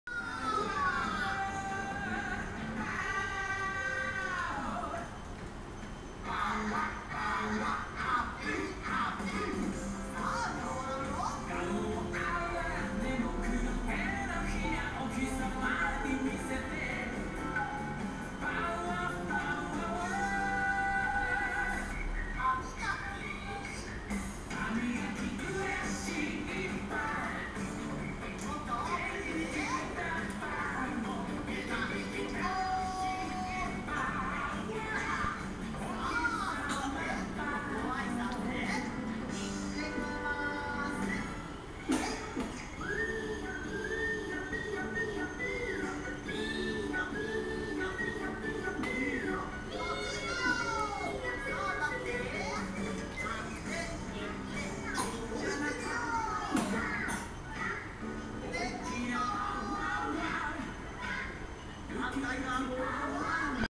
テレビに向かって手を振ります
パパはもちろん、鏡に映った自分の顔にもバイバイします。